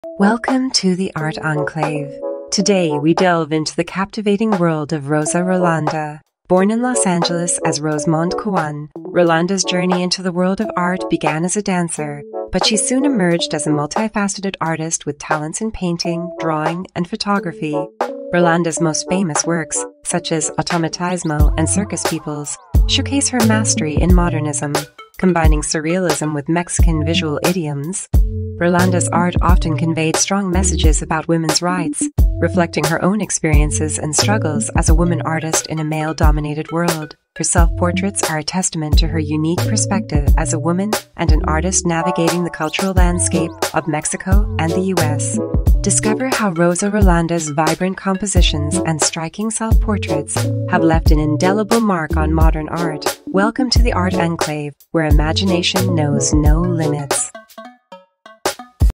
Rosa Rolanda: A Dancer’s Leap into Surrealism and Feminism
Dive into the captivating world of Rosa Rolanda, a multi-talented artist who seamlessly transitioned from acclaimed dancer to innovative painter. In this enlightening video, we explore the vibrant and surreal artwork that sets Rolanda apart in the art world, highlighting her distinctive feminist perspective and the profound influence of her Mexican heritage. Watch as we uncover the rich tapestry of Rolanda's life and work, showcasing her most iconic pieces and the powerful messages they convey.